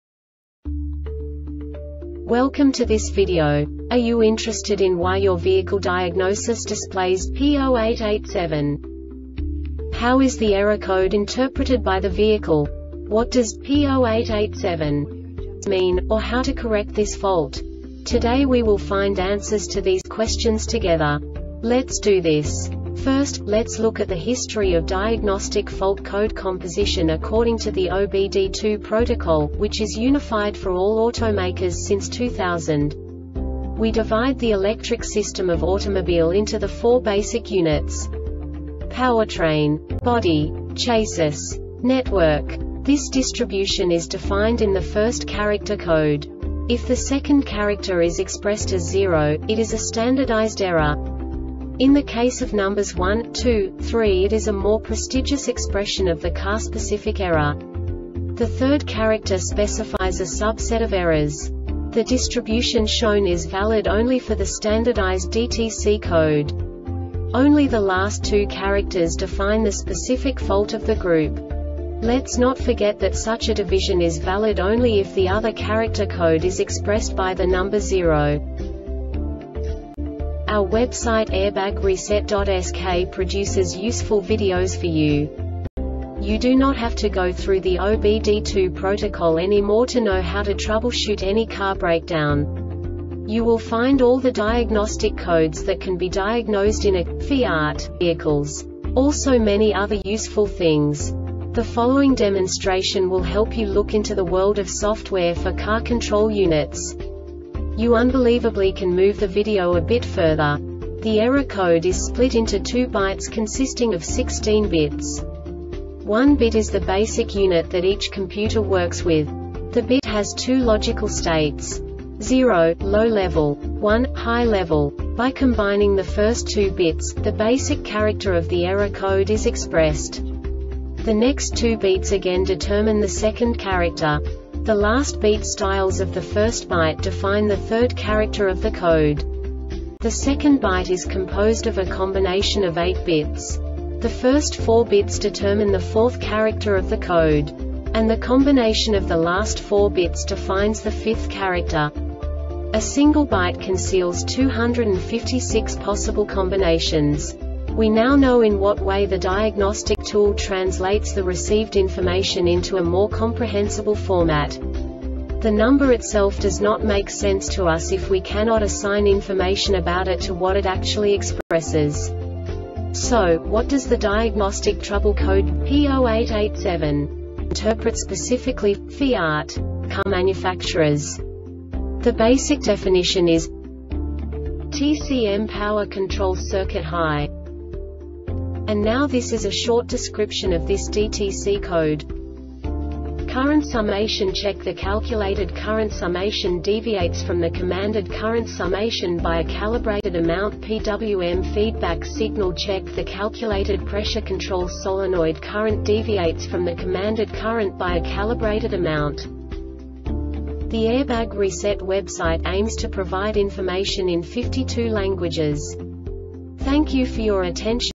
DTC Fiat P0887 Short Explanation
Contents:
0:21 Basic DTC analysis according to OBD2 protocol standard.
1:48 Insight into programming
2:58 A diagnostically understandable description of the Diagnostic Trouble Code
3:05 Basic error definition

Make:
FIAT
Code:
P0887
Definition:
Tcm Power Control Circuit High
Description:
Current Summation Check: The calculated current summation deviates from the commanded current summation by a calibrated amount. PWM Feedback Signal Check: The calculated pressure control solenoid current deviates from the commanded current by a calibrated amount.